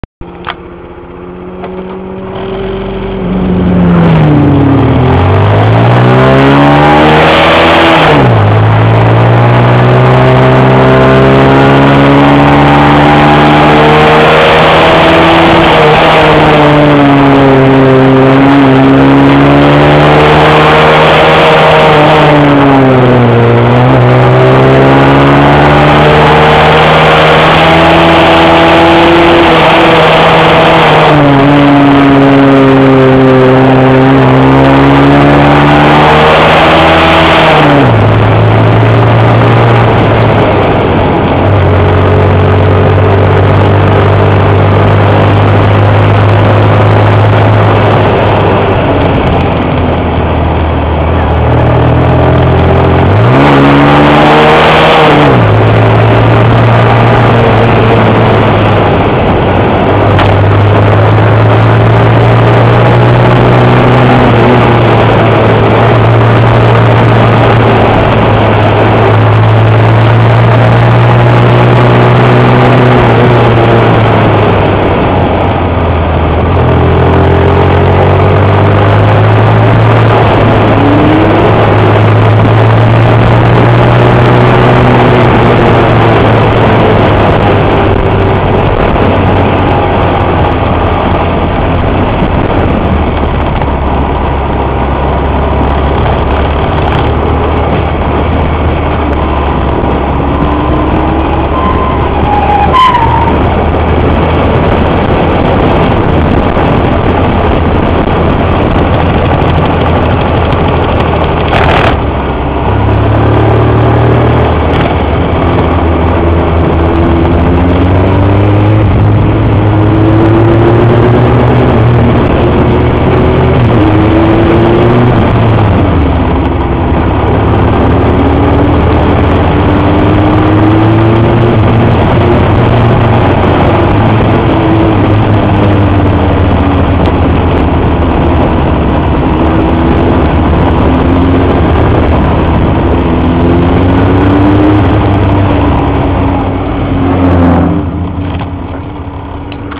カブの待ち伏せ
ビッグバイクをかもろうと待ち構えていたが、少し出遅れた